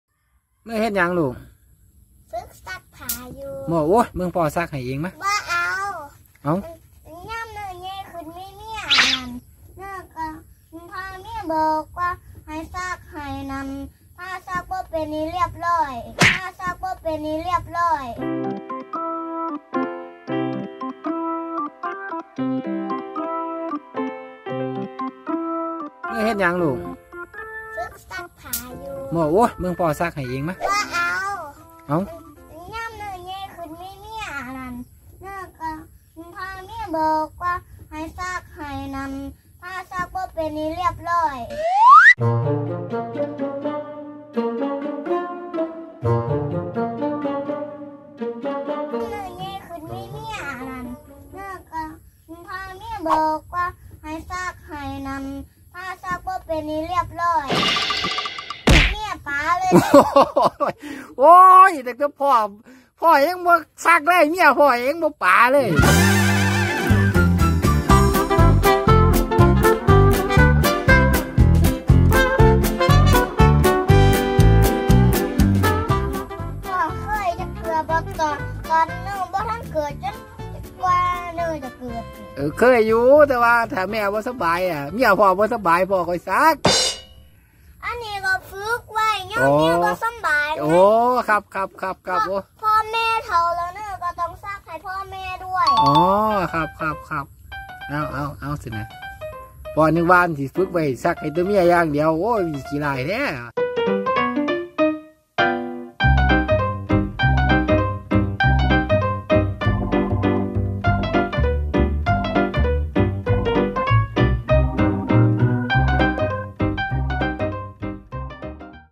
หนูน้อยฝึกซักผ้าด้วยตัวเอง พ่อขอช่วยก็ไม่ยอม พอฟังเหตุผลแล้วต้องอึ้ง คิดได้ไงเนี่ย 😆 : อีเต้ย อีจัน
เด็กน้อยน่ารักฝึกซักผ้าด้วยตัวเอง พ่อขอช่วยก็ไม่ยอม พอฟังเหตุผลแล้วถึงกับอึ้ง! คิดได้ไงเนี่ย 😆